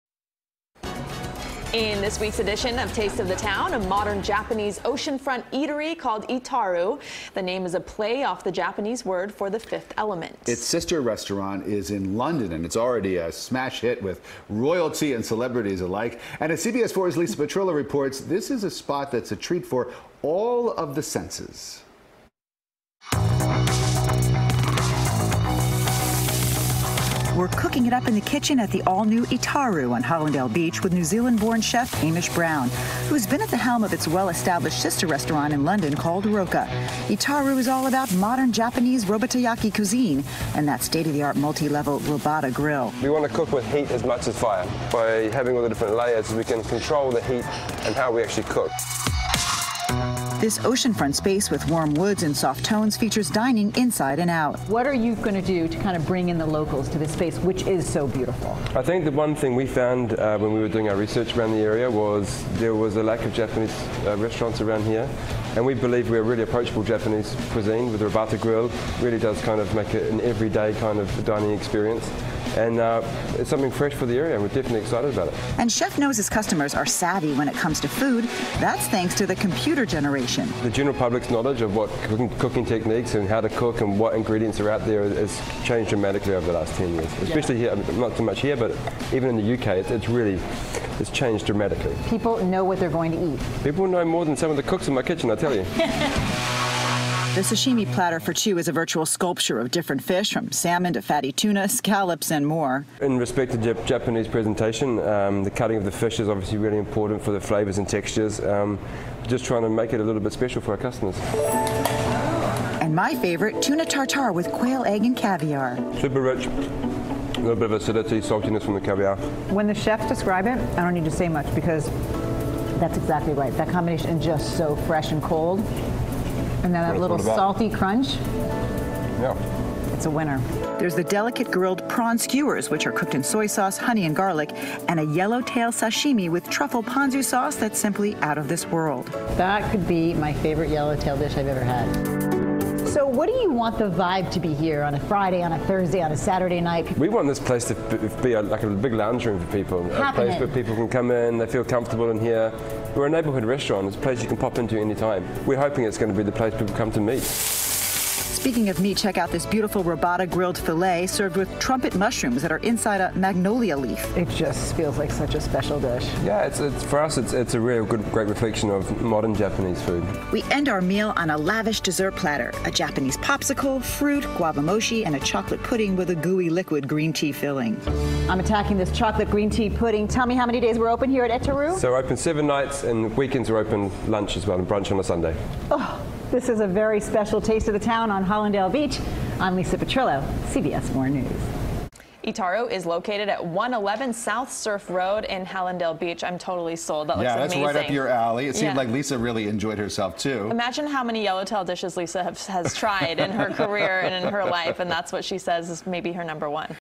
Taste Of The Town: Etaru Brings The Flame To Japanese Cuisine
CBS Miami's Lisa Petrillo reports on Etaru, an amazing Japanese restaurant in Hallandale Beach.